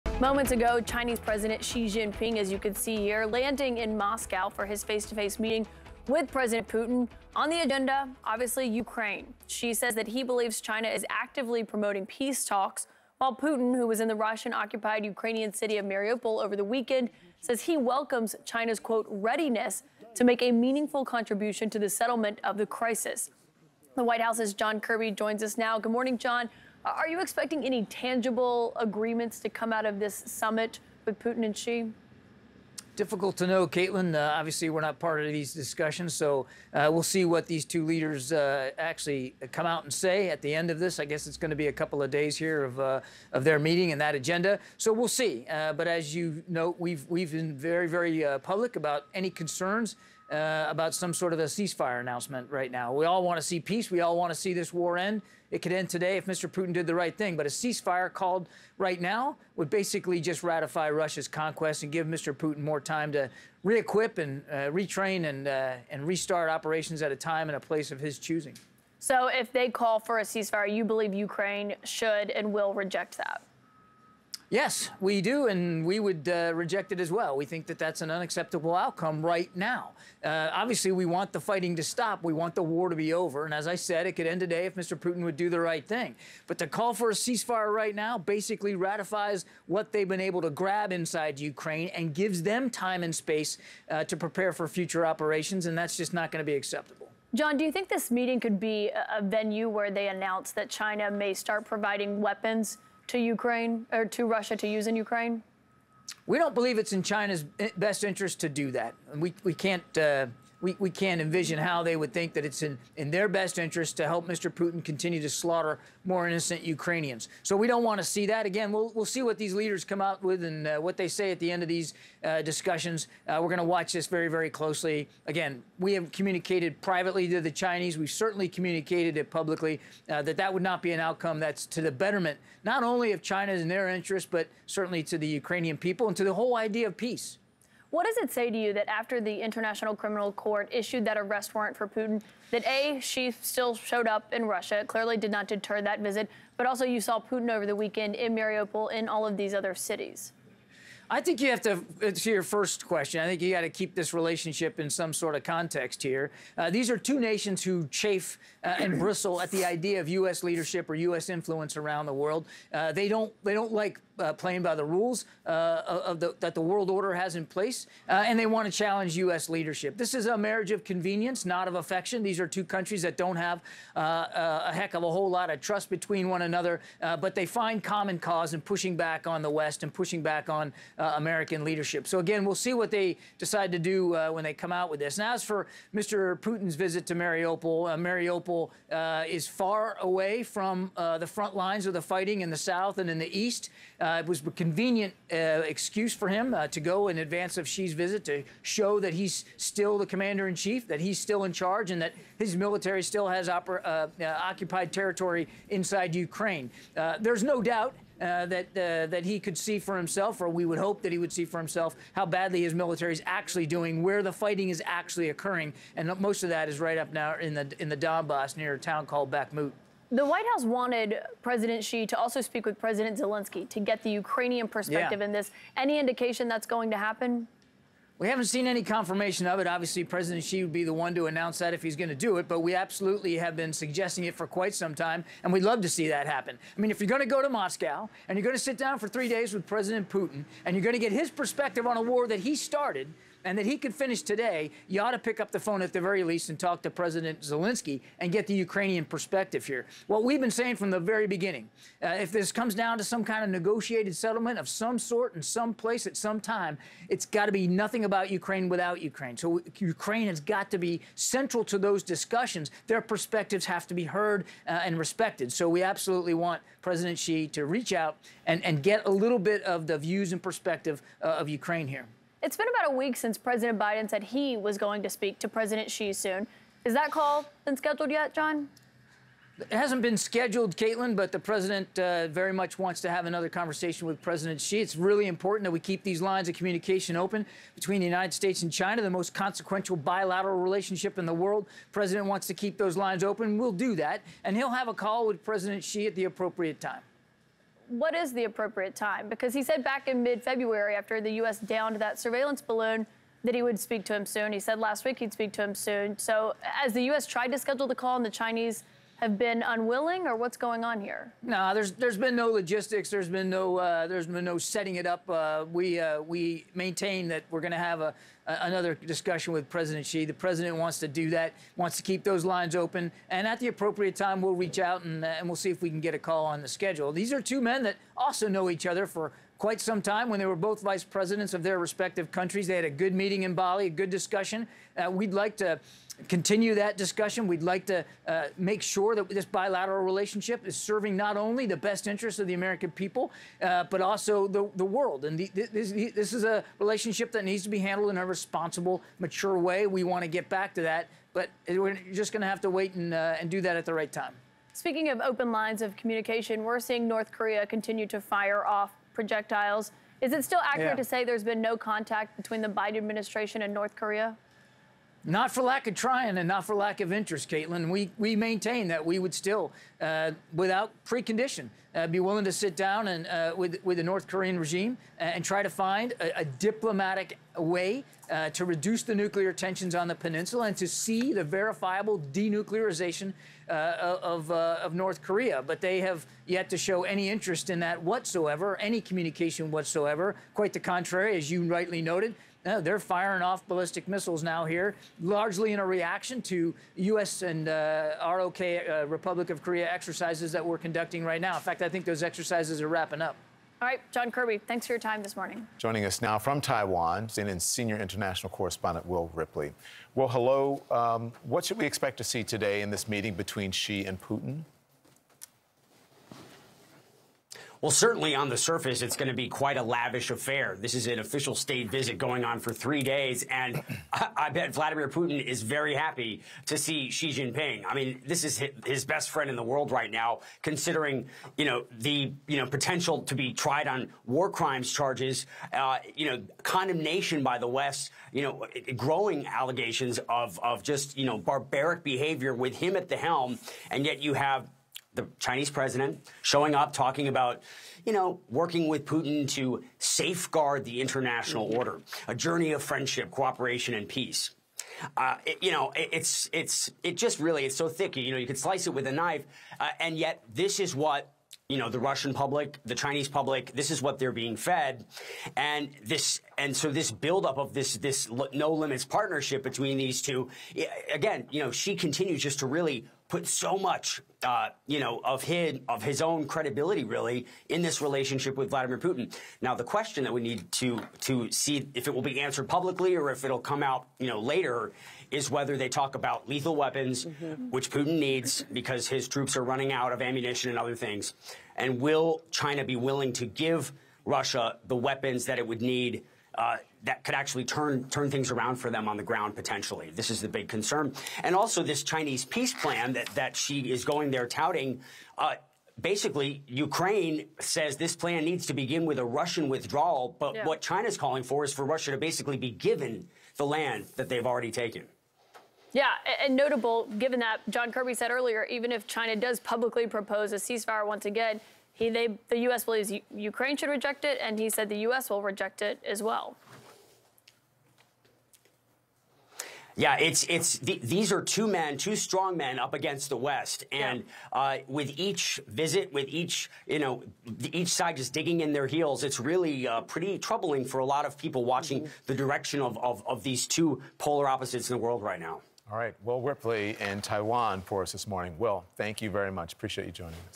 ‘A marriage of convenience not affection’: Kirby on Putin-Xi alliance
National Security Council spokesman John Kirby discusses the alliance between Russian President Vladimir Putin and Chinese leader Xi Jinping.